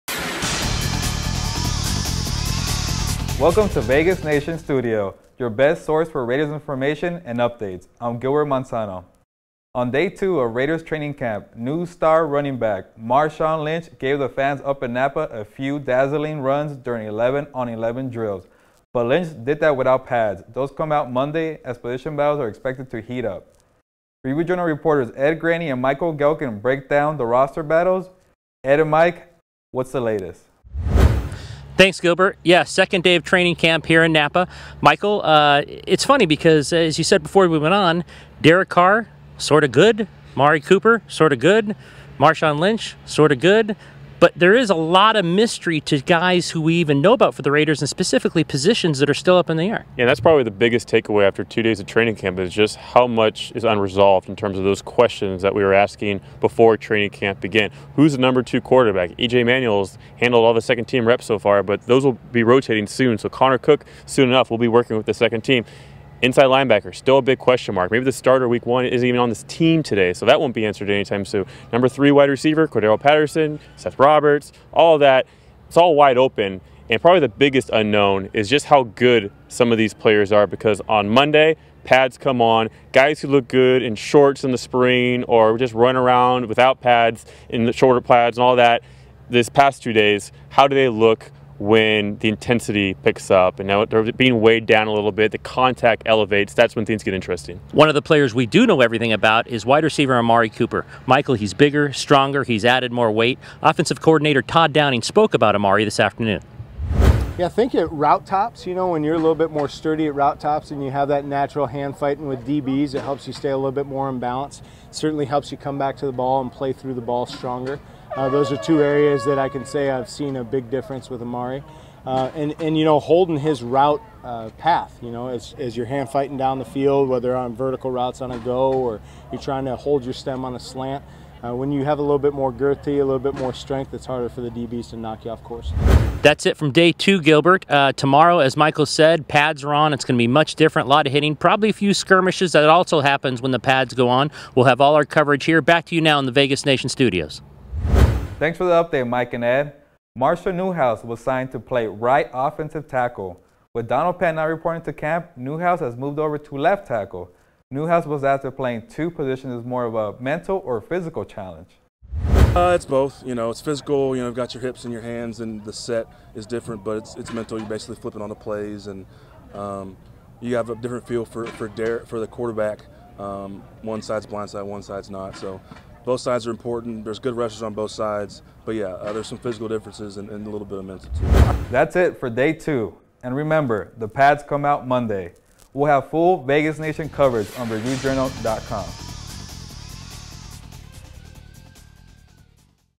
Raiders Training Camp Day 2
On Day two of Raiders training camp, Marshawn Lynch gave the fans up in Napa, California, a few dynamic runs, and Marshall Newhouse discussed the challenges of playing at both offensive tackle spots. With the pads coming out Monday, Review-Journal reporters Ed Graney and Michael Gehlken breakdown the position battles, including the No. 2 backup quarterback job. (Las Vegas Review-Journal)